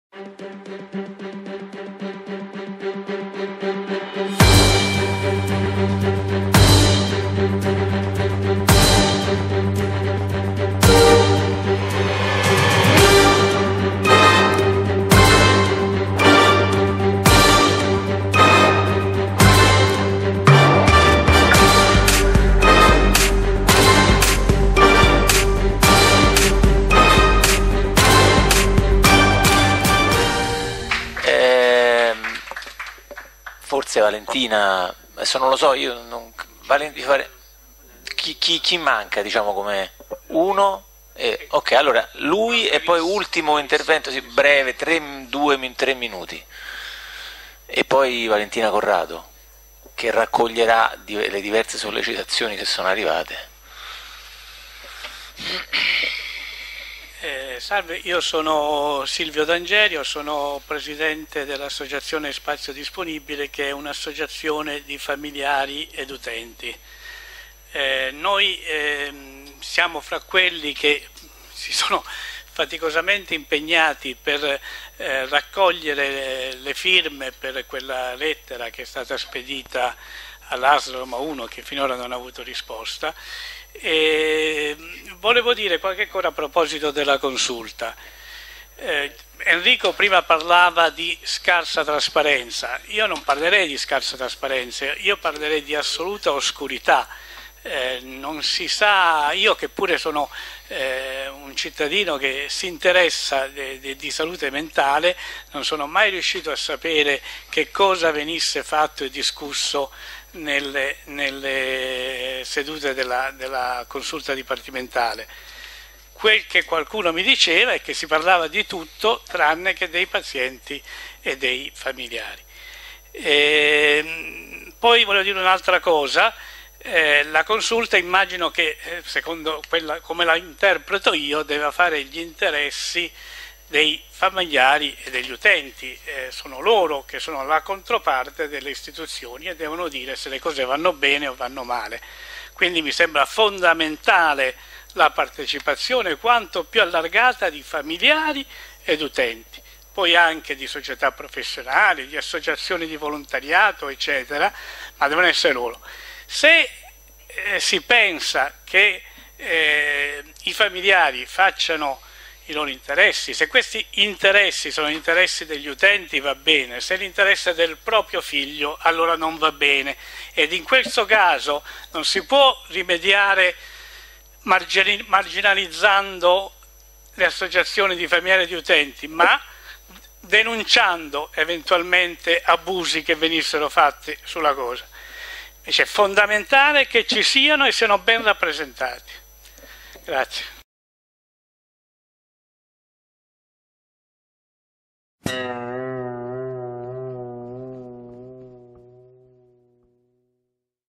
Tavolo regionale sanità m5s: 5 proposte per la sanità del Lazio. Interventi pubblico: S. D’Angerio
Registrazione effettuata da liberi.tv a Roma il 17 giugno 2017

Tavolo M5S Sanità regionale del Lazio - 5 Proposte da cui partire per riformare la sanità del Lazio

1- Il bilancio e gli strumenti di rendicontazione e controllo del S.S.R. tra servizi pubblici e sanità privata
2-Sanità pubblica e strumenti partecipativi
3-Strutture ospedaliere e servizi territoriali di prossimità
4-Integrazione socio sanitaria
5-Cultura della salute mentale

Intervento di Silvio D'Angerio , presidente dell'associazione Spazio Disponibile